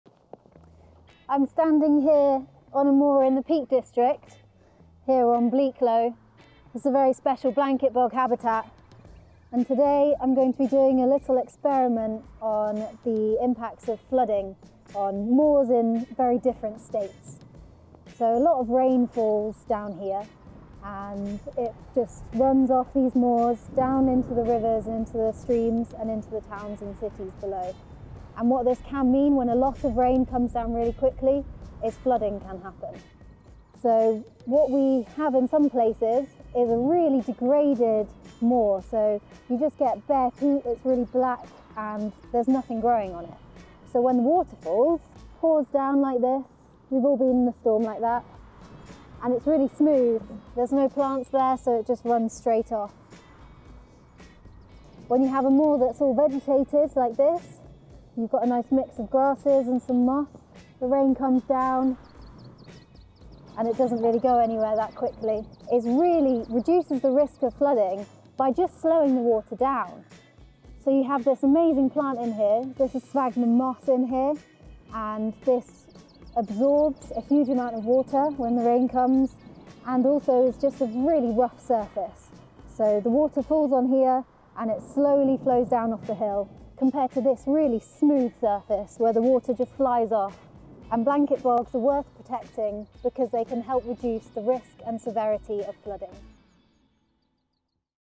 Blanket bogs are worth protecting – Reducing the risk of flooding
It rains a lot on the moors of the Peak District and South Pennines. The unique plants that grow on healthy blanket bogs slow the flow of rain from the moors to the towns and cities below.
Mollie from Moors for the Future Partnership carries out an experiment on Bleaklow in the Peak District to show us how these landscapes can reduce the risk and severity of flooding.
Blanket bogs are worth protecting because they can help reduce the risk and severity of flooding